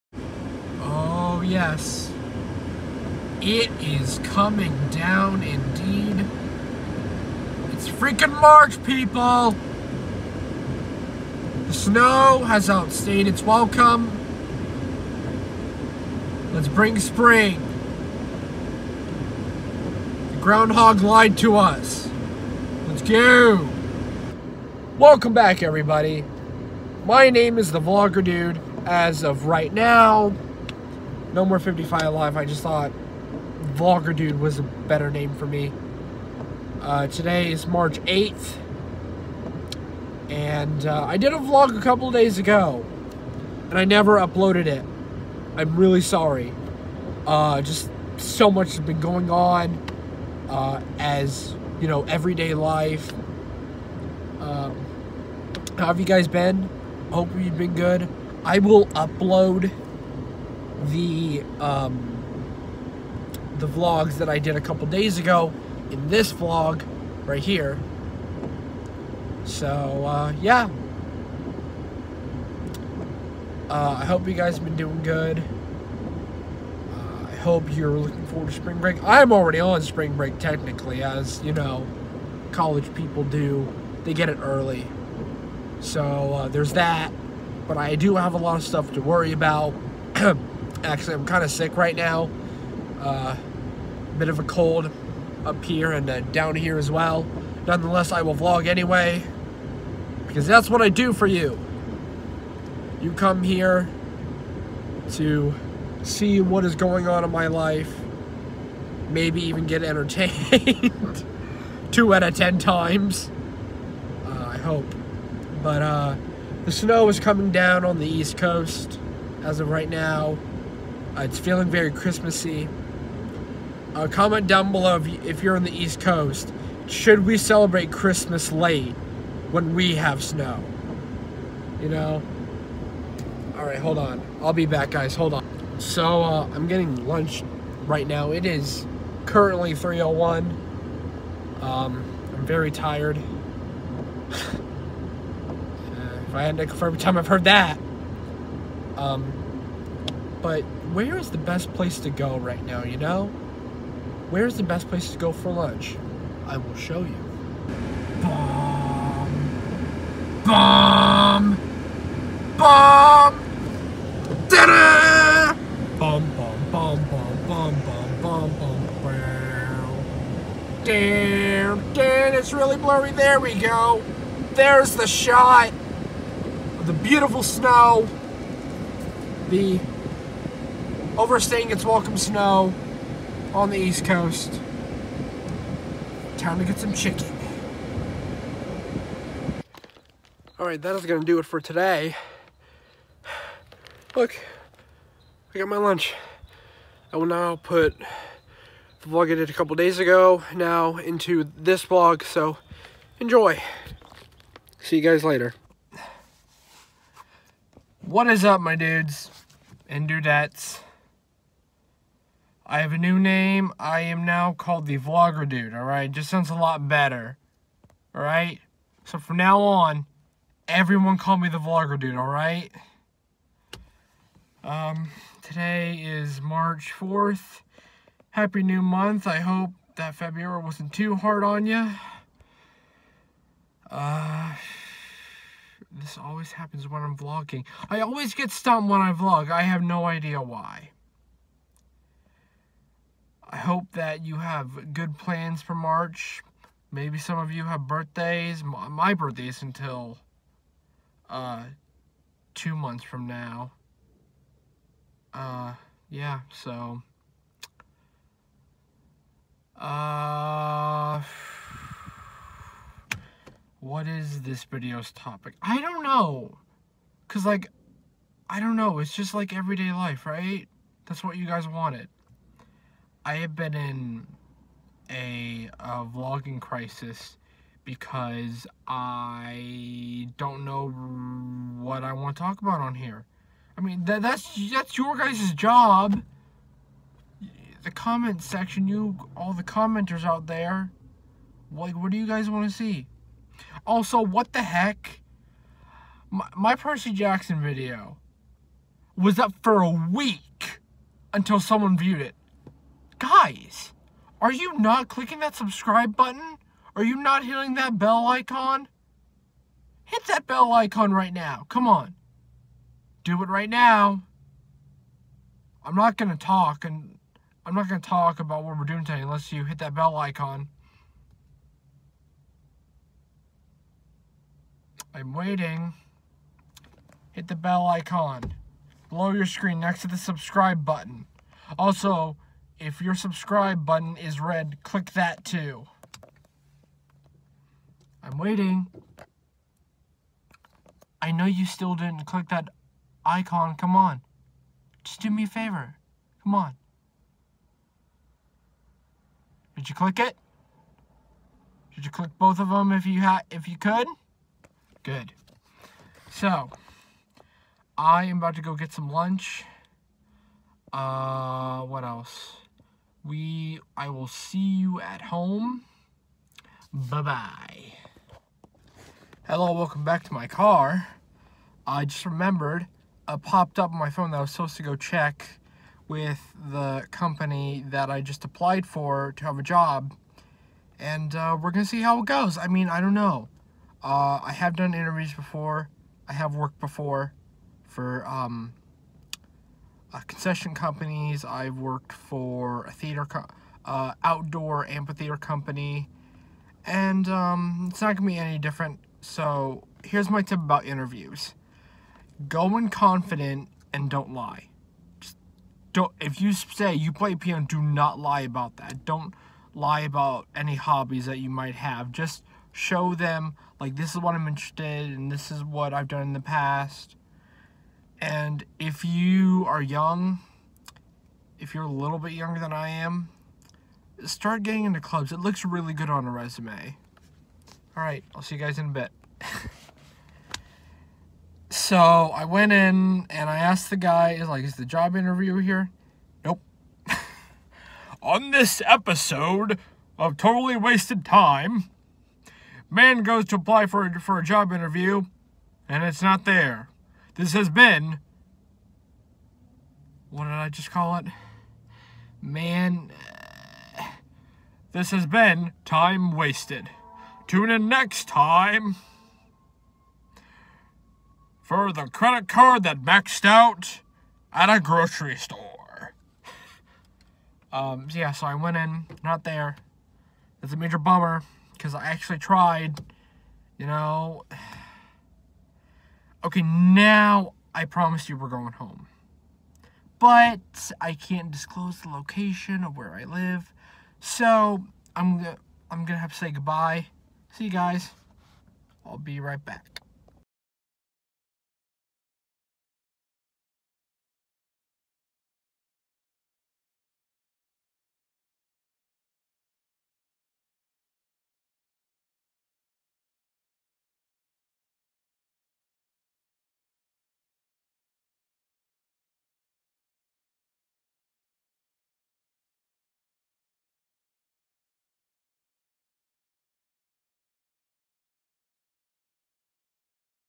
The Vlogger Dude #9: SNOWING in MARCH?!!
Jack Frost snuck up on us here in the east coast today. Wonder why? Also thank you guys so much let’s keep this channel growing!